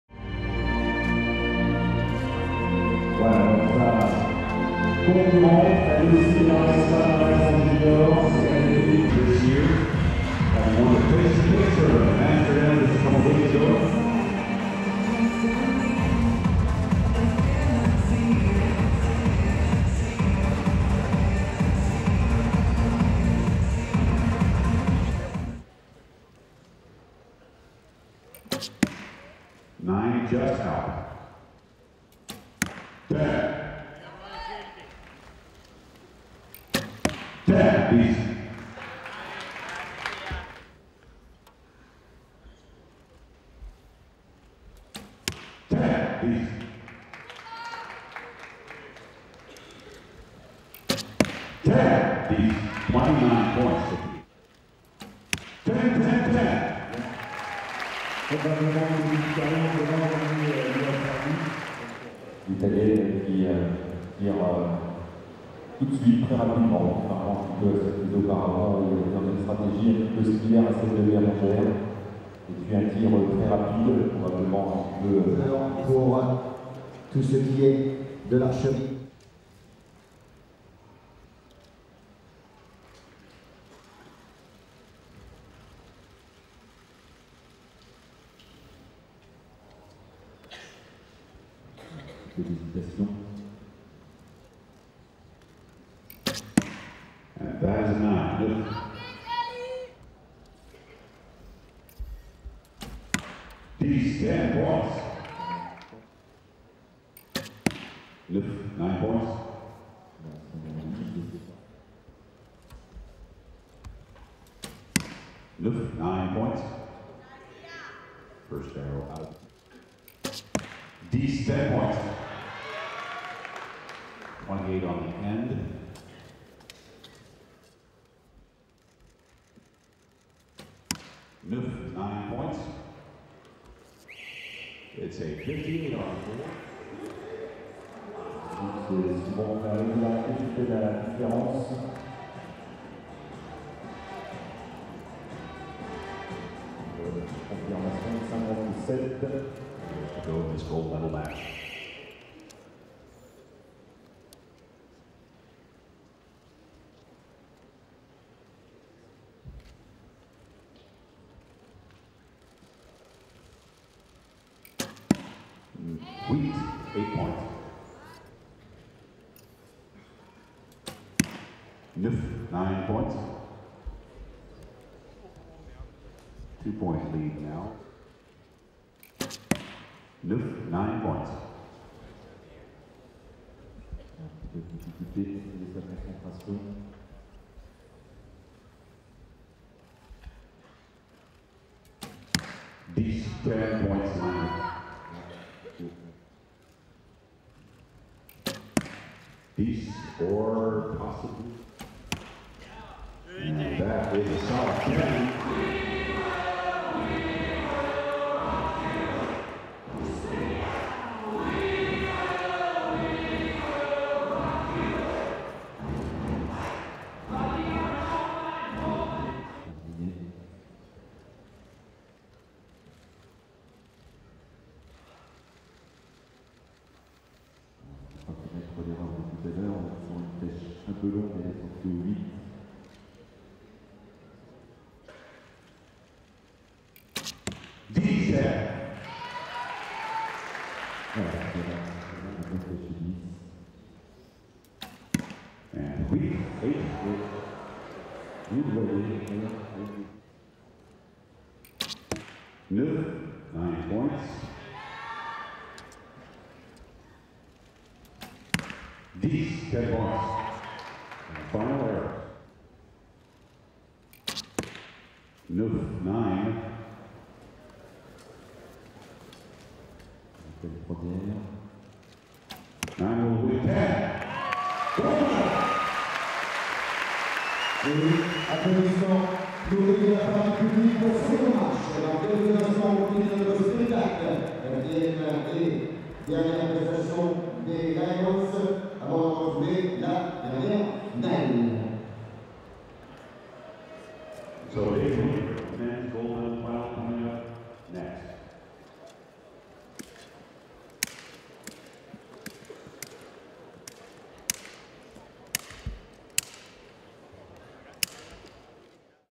12th European Tournament of archery 2009 - Ind. Match #2
Uncut Match of the 2009 European Tournament of archery in Nimes (FRA).
Recurve Women Ind. Gold / P. LIONETTI (ITA) vs E. RICHTER (GER)